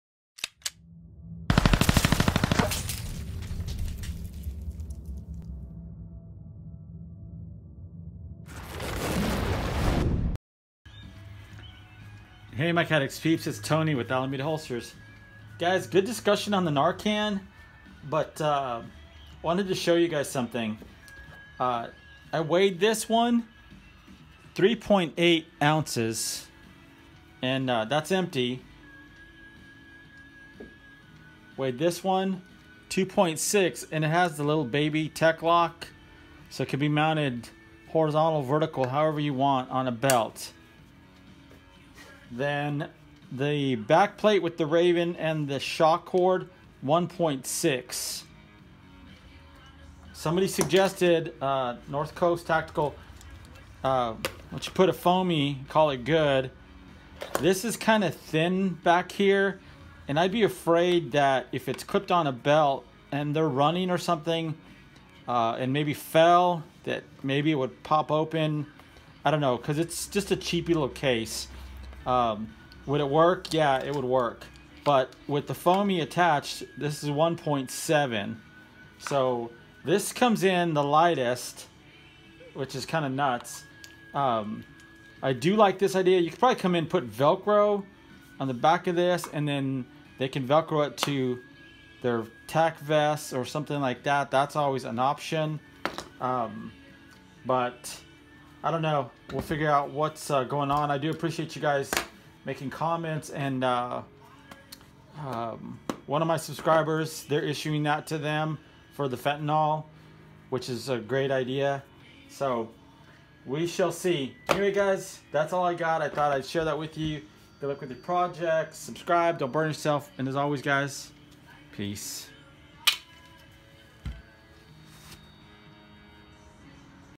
Weight- N WORD
YouTube isn’t a fan of the word Narcan, anyway I took a minute to weight all three Holsters.

Plastic is thin on that Narcan case, not sure if a FOMI would cut it during extreme conditions.

“Don’t burn yourself”

All content produced by A.C.H. are 100% unbiased reviews and opinions.

Follow A.C.H.